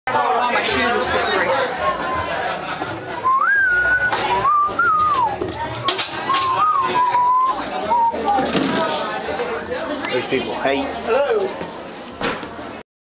Bube's Brewery: Into the catacombs
Bube's Brewery: Down the old stairs and into the catacombs, where we find elf-costumed employees cleaning up after a Christmas feast.